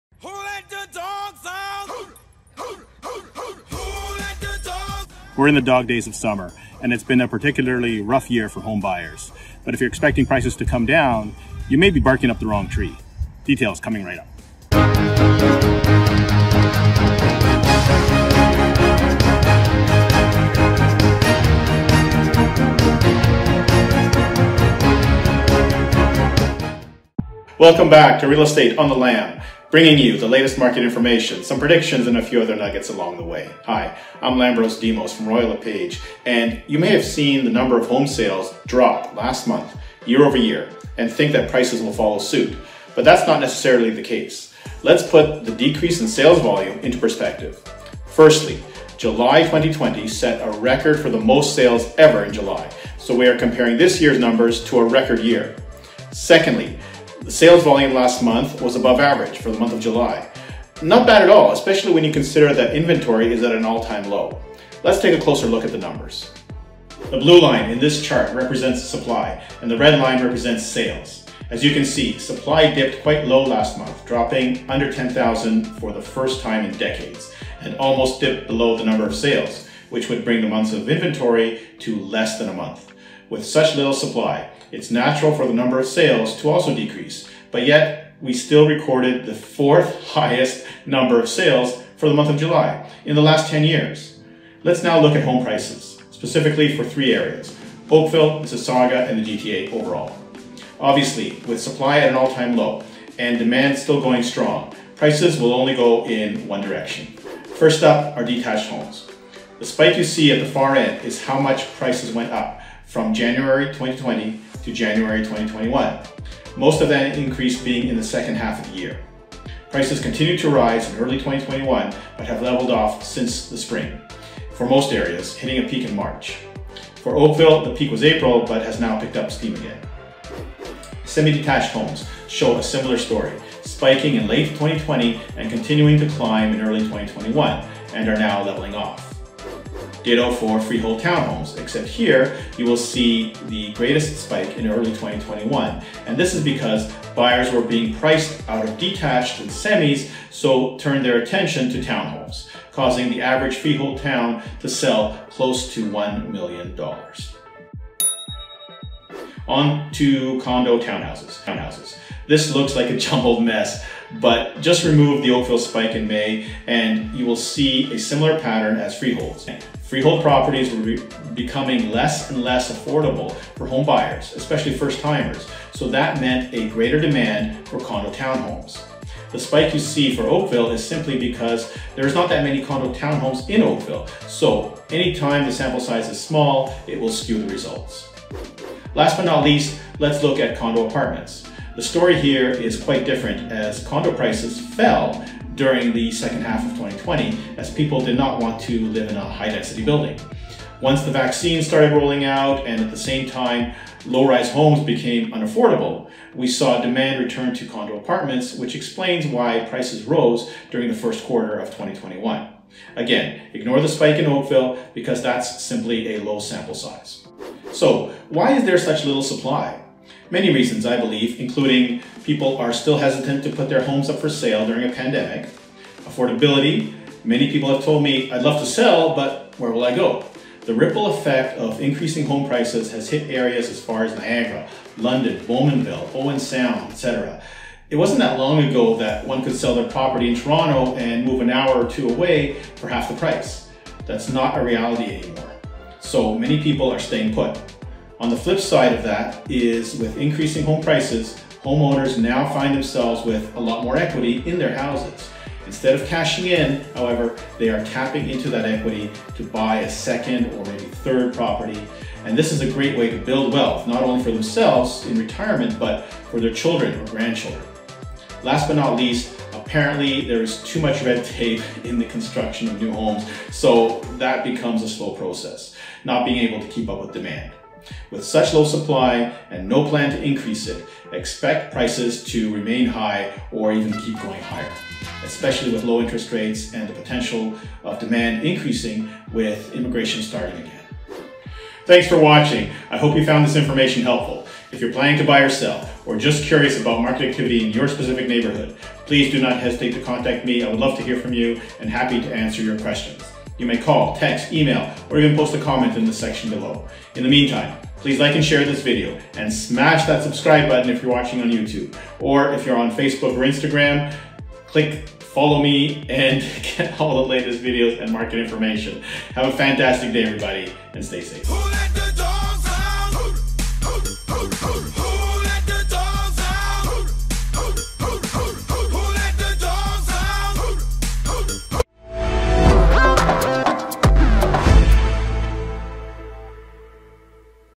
GTA Real Estate Update August 2021 | Real Estate on the Lam
🐕 The dog days of summer are here and that means real estate activity is slowing down - like everything else - so if you are looking to buy, take advantage of this slower time because competition will likely heat up again as the weather cools down. Watch video for details!

Quick reference:
00:00​​ Introduction
01:06 Inventory and sales numbers for the month of July from 2012 to 2021
01:34​​ Home prices by property type (Jan 2020 to July 2021)
03:37 Reasons for low supply
04:37​​ Closing remarks / contact information

Meet Me in Mississauga
Lambros Bryan Demos